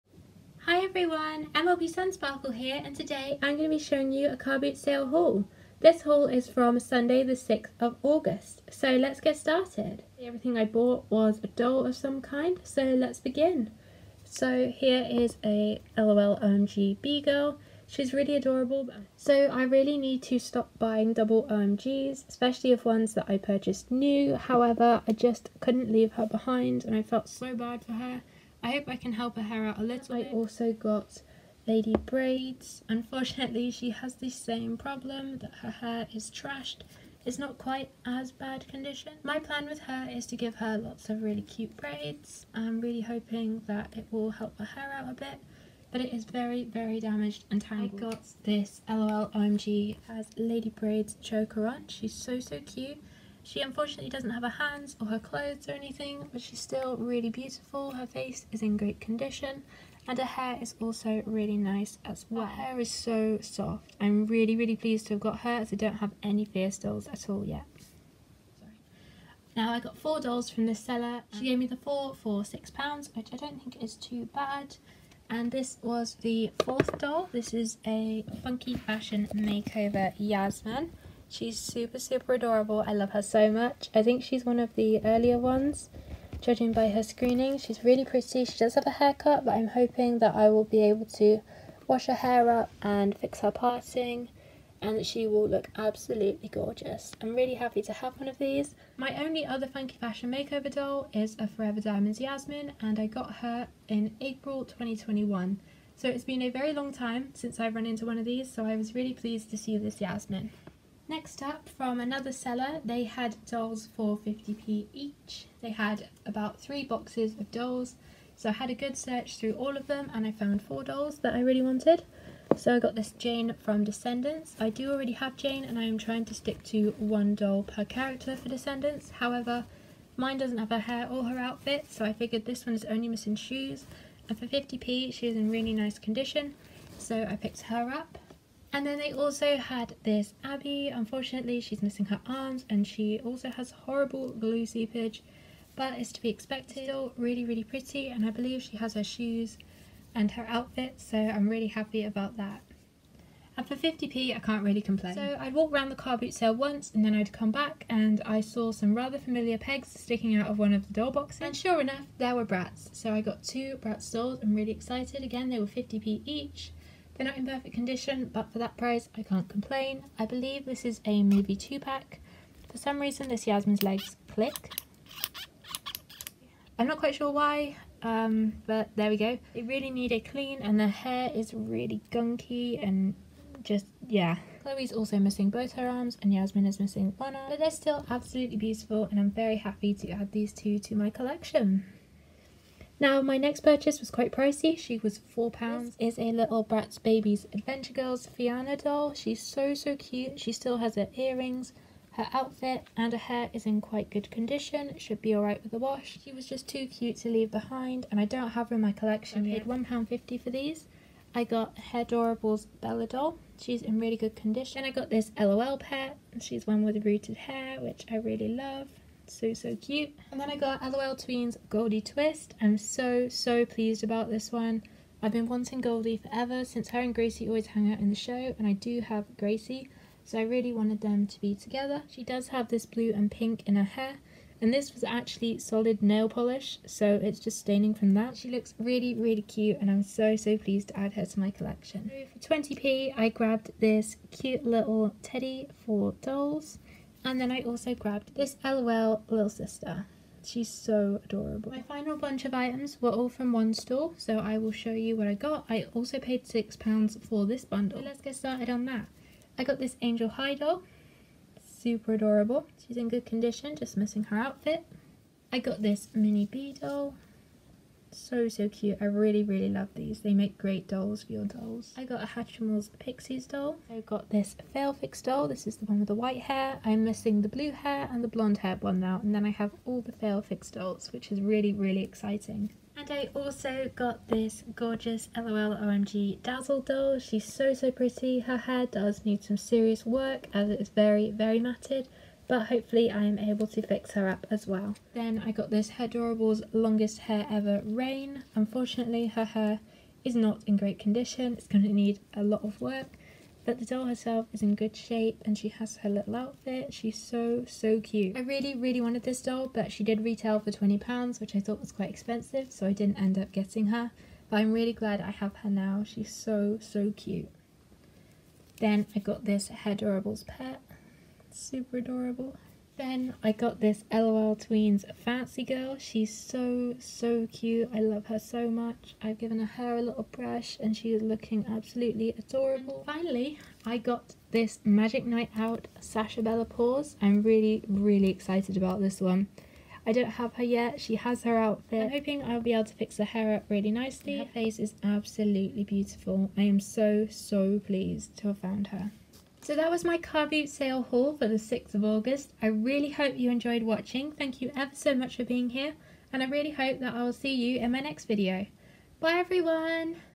Sunsparkle’s Car Boot Finds! Doll Haul! Bratz, Monster High, LOL OMG, Bratzillaz + More!
Hi everyone! Today Im showing you what I got from the car boot sales on the 6th of August! I got a LOT of dolls today!